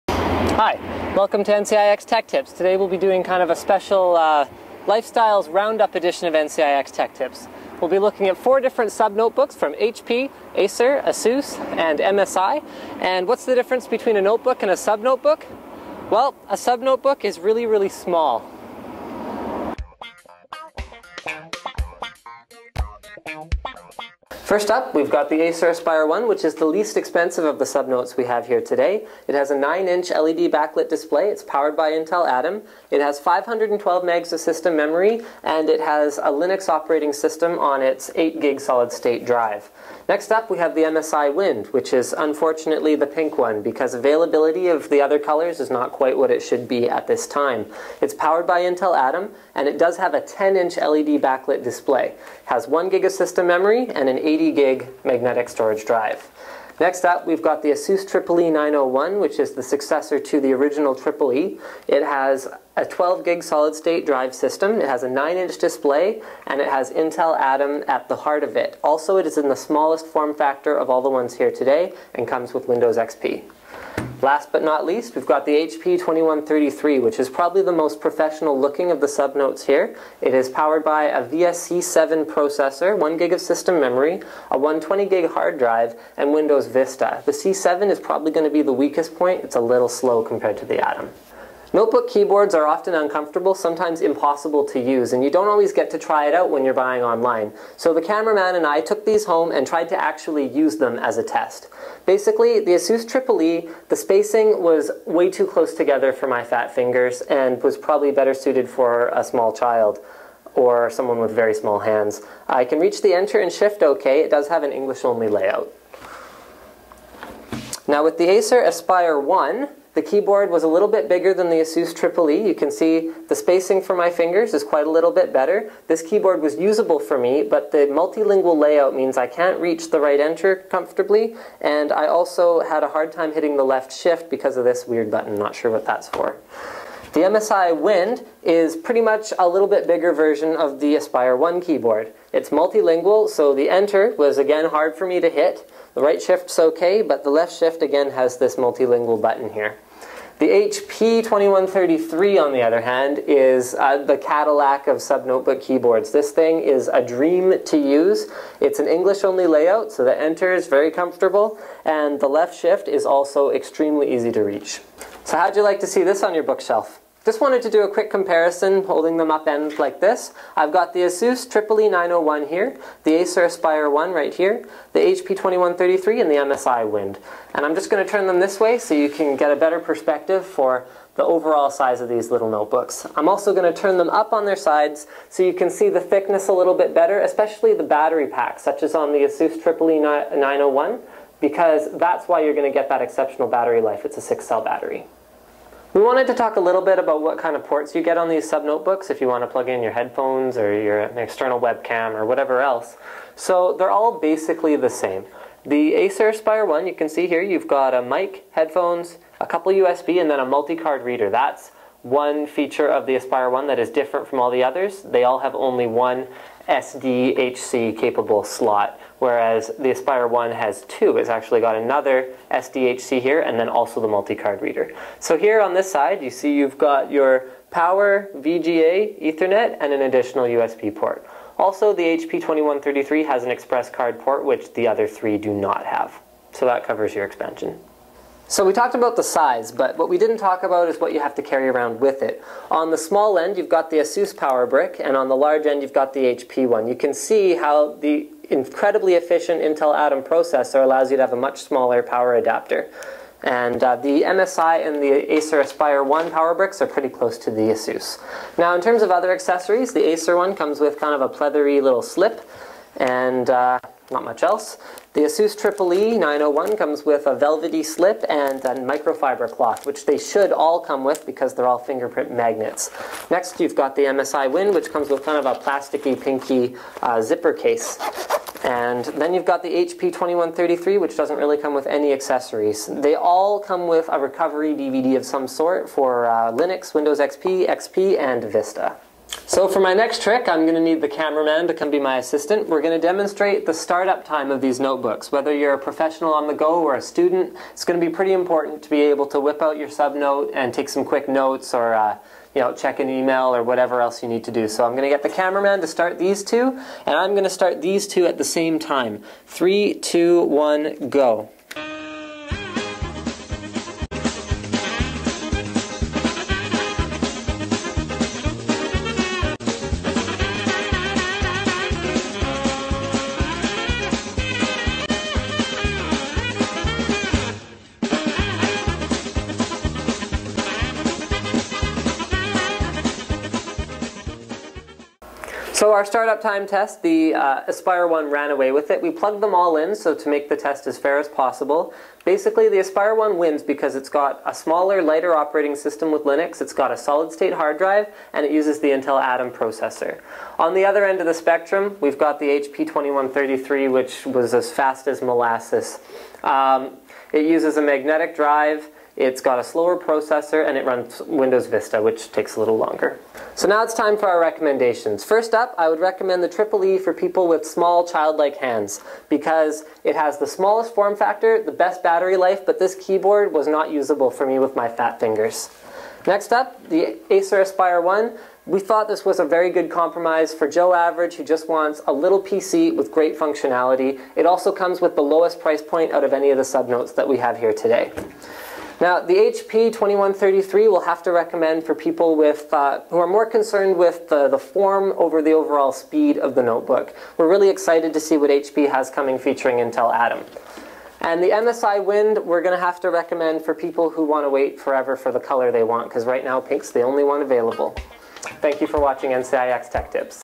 Acer Aspire One, Asus Eee 901, HP 2133, MSI Wind Hands On (NCIX Tech Tips #16)
In Tech Tips number 16, Linus will show you the hands on experience on all the latest Intel Atom sub-notebooks from Acer, ASUS & MSI.  Linus will also compare them with the VIA C7-M powered HP2133 sub-note.